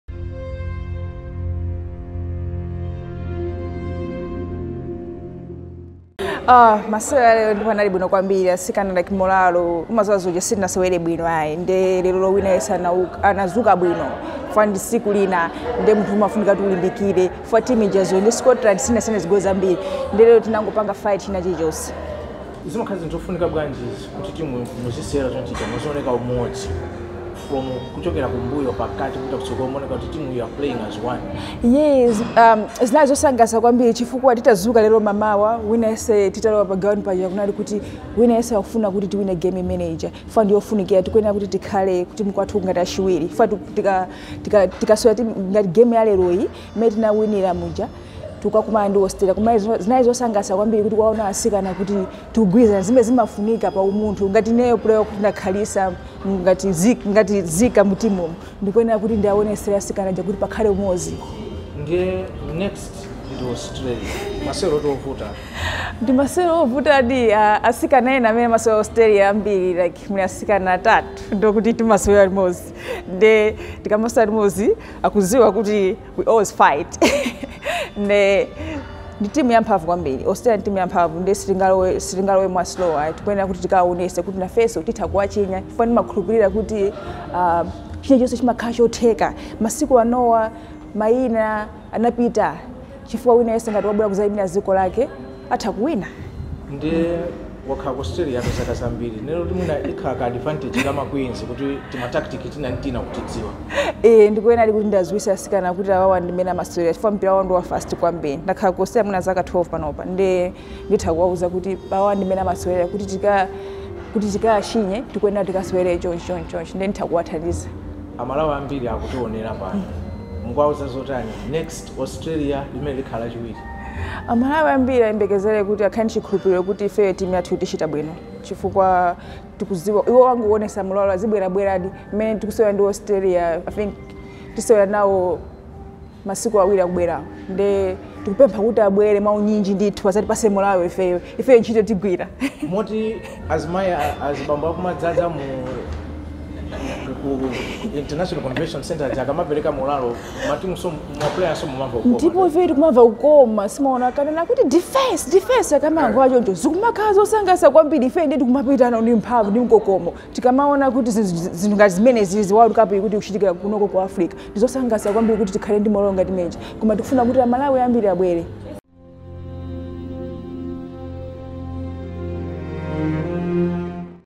Mwai Kumwenda speaks to WaGanyu on today's victory over Barbados.
Mwai Kumwenda speaks to WaGanyu on today's victory over Barbados and the retun of the spirit of united in the Queens camp after Saturday's infighting.